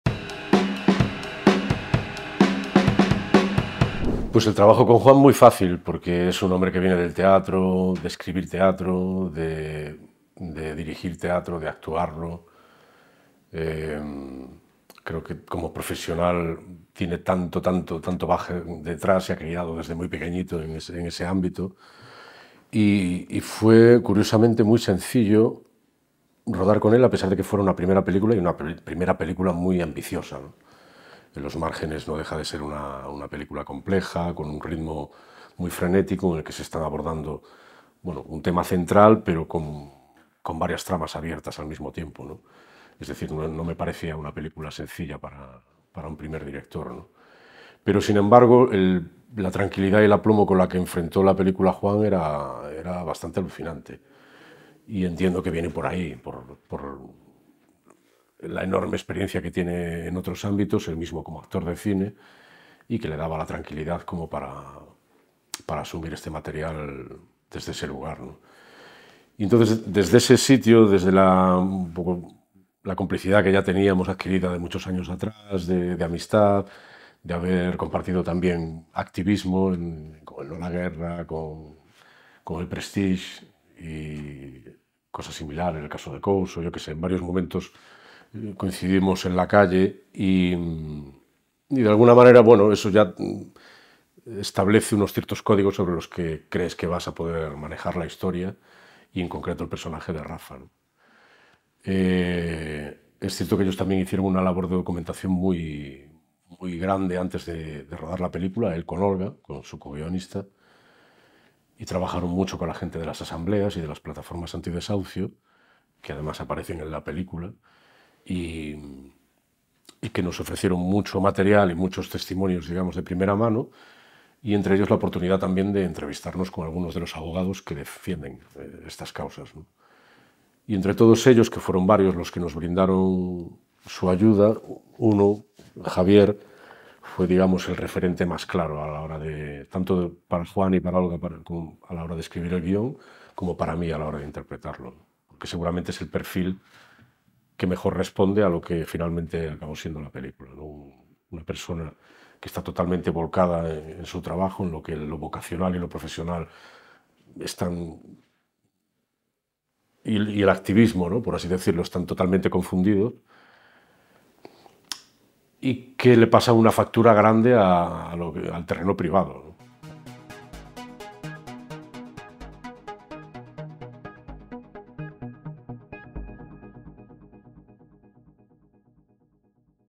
Entretien avec Luis Tosar, acteur de "En los márgenes"
Entretien avec Luis Tosar, acteur de "En los márgenes / A contretemps" (2022), pendant la 32e édition du Festival du Cinéma Espagnol de Nantes

Entrevista con Luis Tosar, actor de "En los márgenes" (2022), durante la 32.ª edición del Festival de Cine Español de Nantes

Interview : Sonia Fernández Hoyos
Traduction : Mélanie Doré
Image : Bastien Capela & Emmanuel Larue